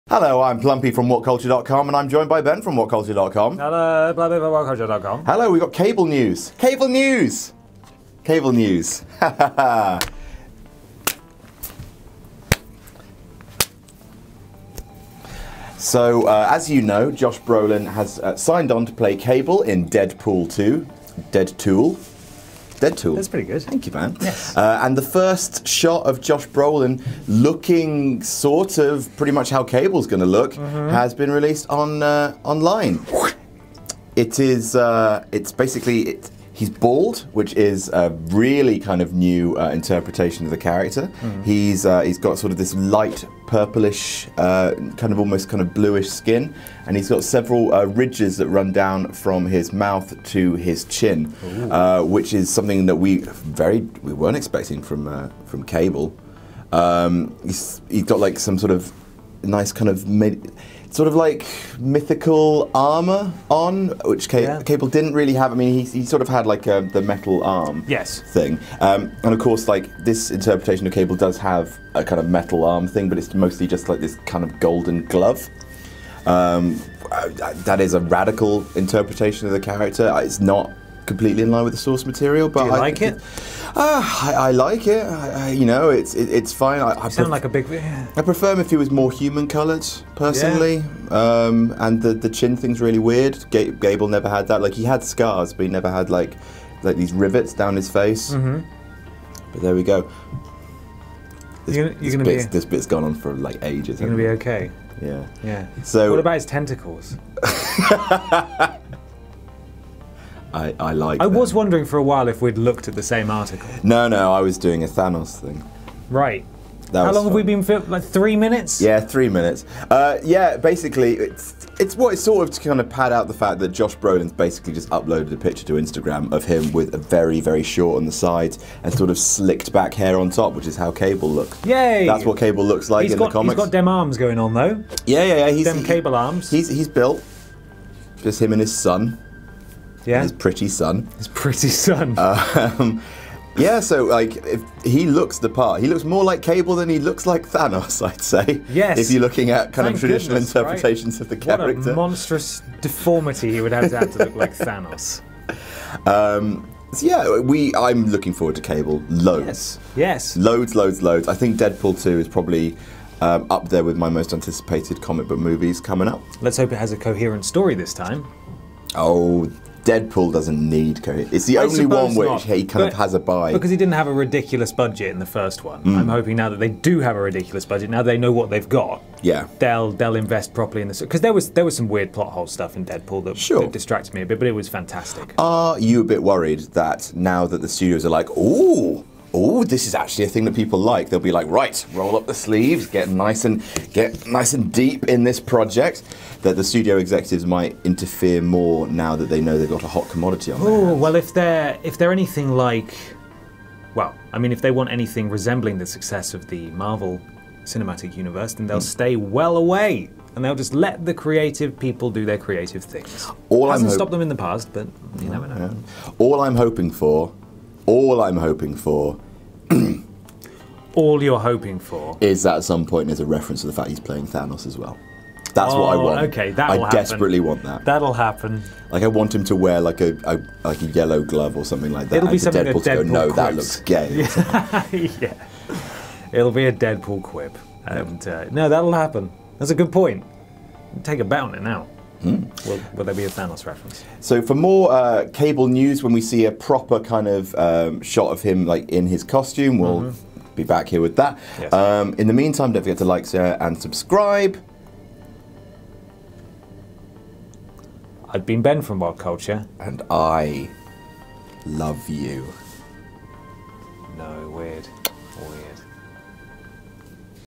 Josh Brolin Reveals His Deadpool 2 Cable Haircut
He's starting to really look the part...